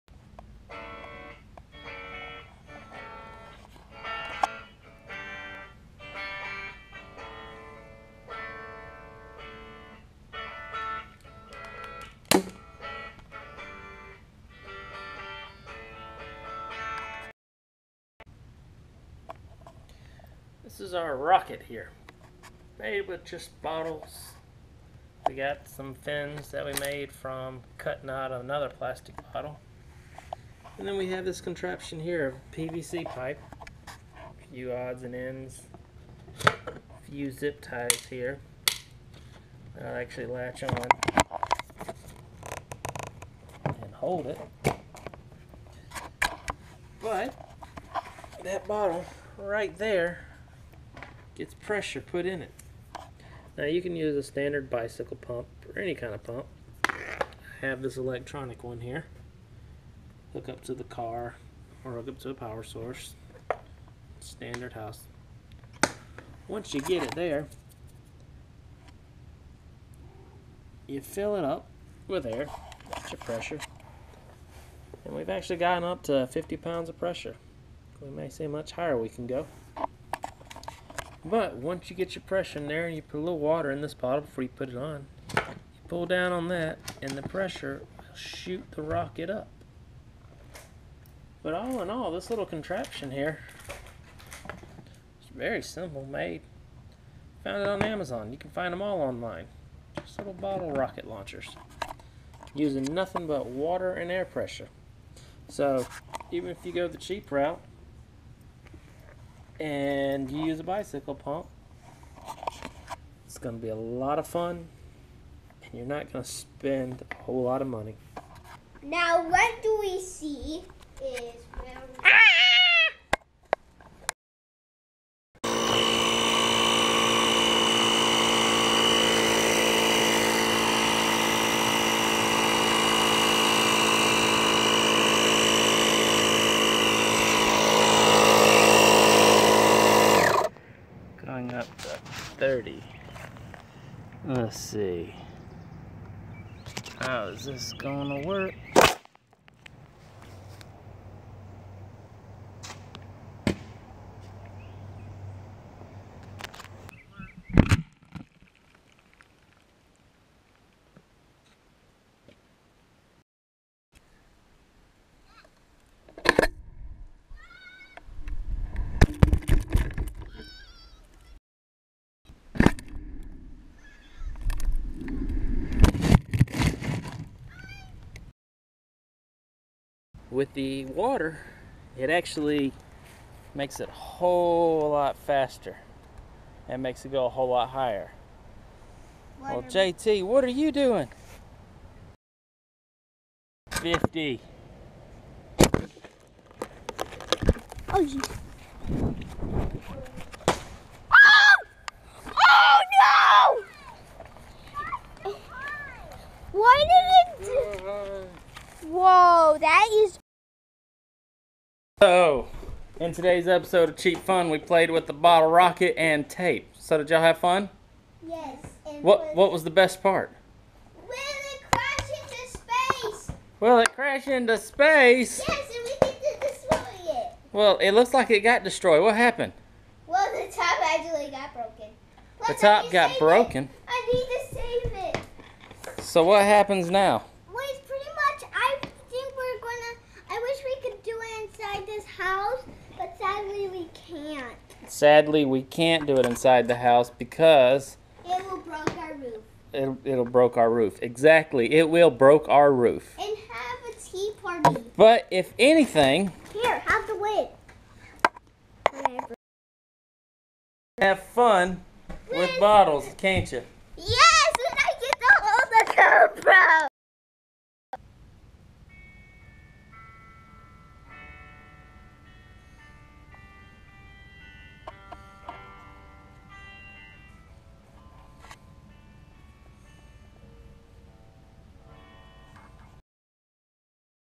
Cheap fun with recycle bottle rocket.
Using recycled bottles to have plenty of fun